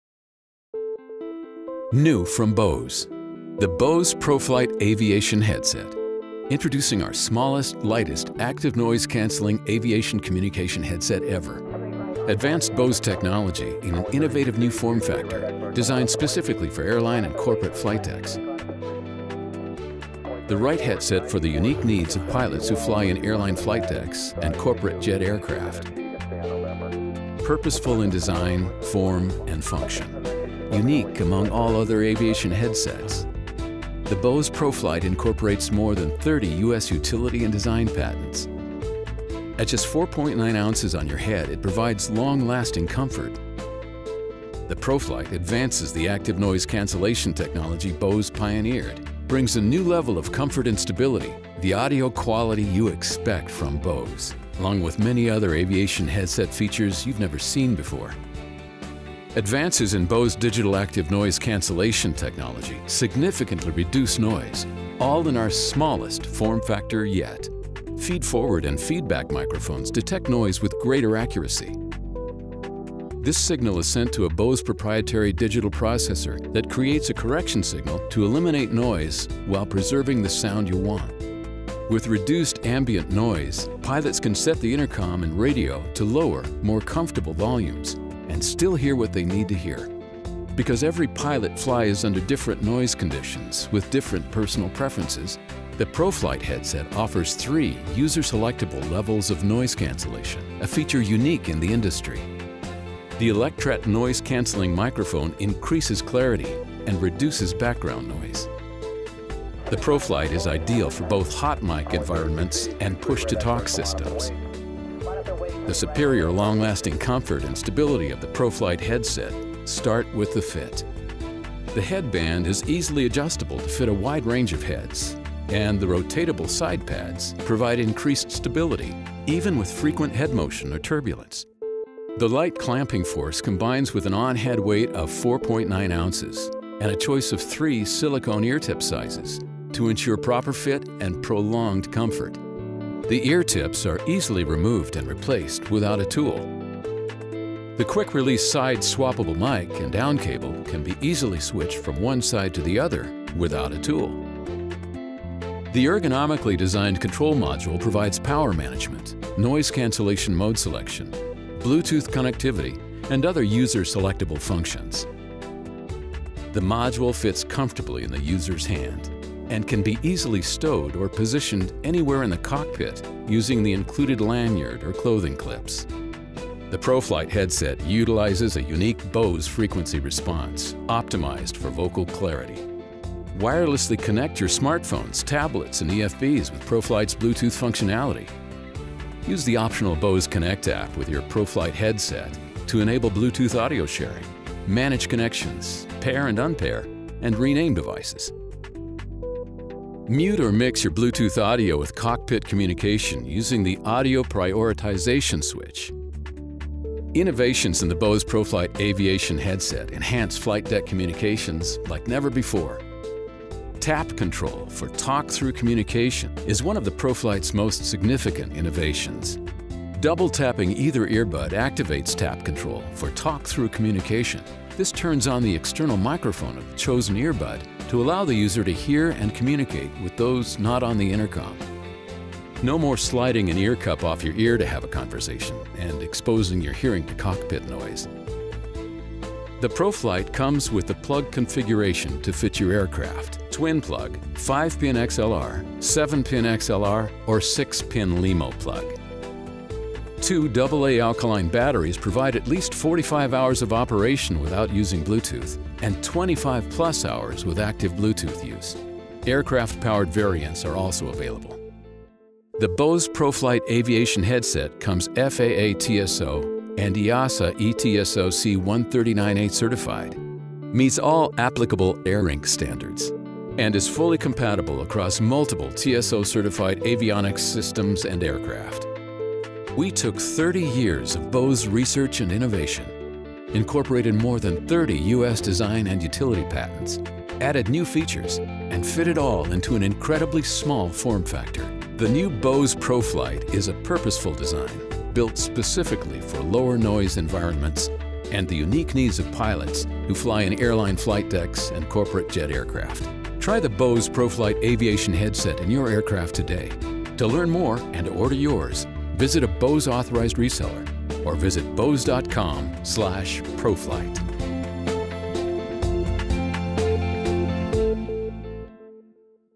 Bose | Introducing the ProFlight Aviation Headset
The Bose ProFlight Aviation Headset is the industry's smallest, quietest, and most comfortable active noise cancelling communication headset. With an on-head weight of 4.9 ounces, the new Bose ProFlight Aviation Headset is designed for long term comfort and is our most innovative headset yet.

Engineered for pilots of moderately noisy pressurized turbine powered aircraft, it incorporates more than 30 U.S. utility and design patents. The ProFlight Aviation Headset features three levels of user selectable active noise cancellation, active equalization to improve intelligibility of incoming transmissions, a noise cancelling microphone for clearer outgoing transmissions, talk-through tap control for communication with those outside the intercom, and full function Bluetooth® connectivity with smartphones, tablets or EFBs. The ProFlight Headset is FAA TSO and EASA E/TSO-C139a certified, meets all applicable ARINC standards, and is available with four different plug configurations.

Visit Bose.com/ProFlight to learn more.